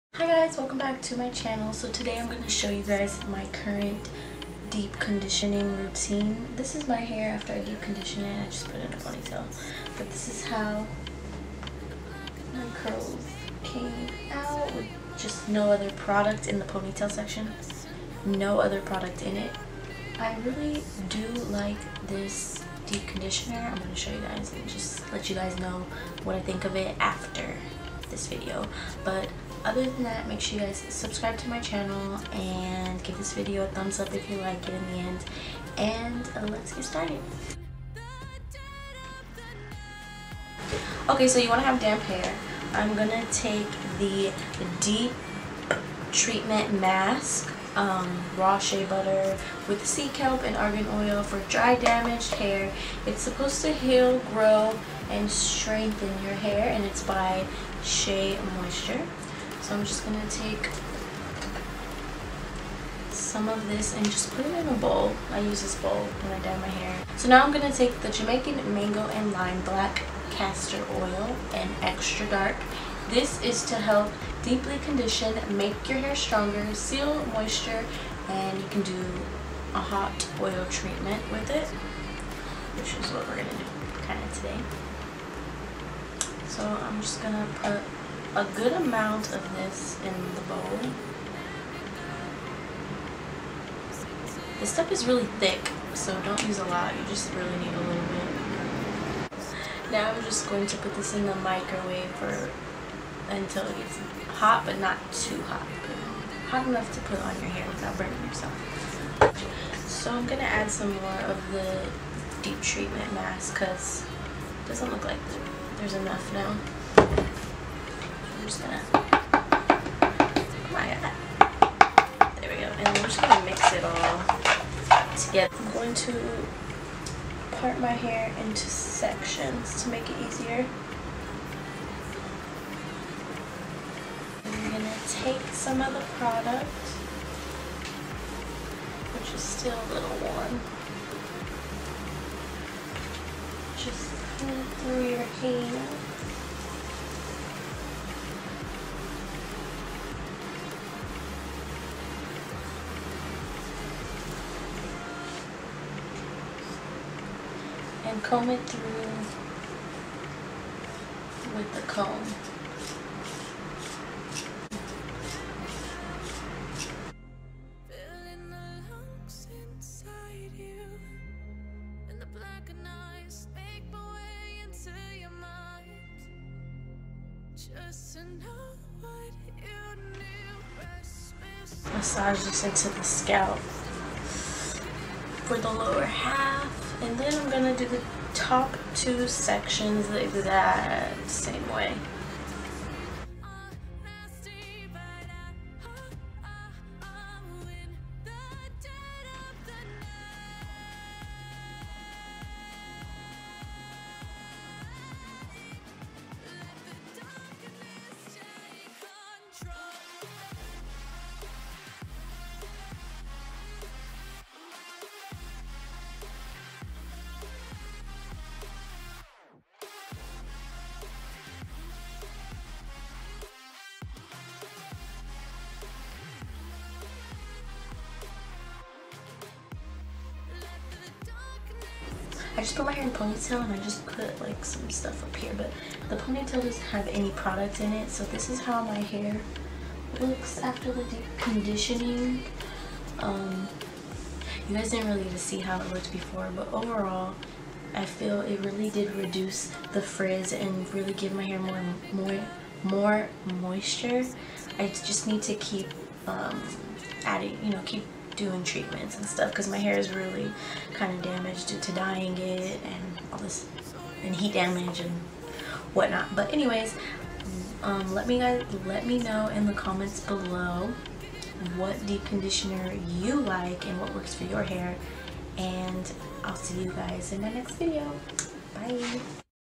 Deep Conditioning
Hey Yall!

My current deep conditioning routine! I do this once a week to eliminate frizz and make my hair healthier, shiny, grow, and stronger. Its important to deep condition curly hair once a week. This is a huge hair tip and trick! I recently am on the hunt to find new hair products that add moisture to my hair to control frizz and define my curls making them look shiny and healthy!

Products Used:
1. Shea Moisture Raw Shea Butter Deep Treatment Masque
2.Jamaican Black Castor Oil Mango and Lime
*Both products were purchased at target

Instagram & Twitter: @CroatianBishh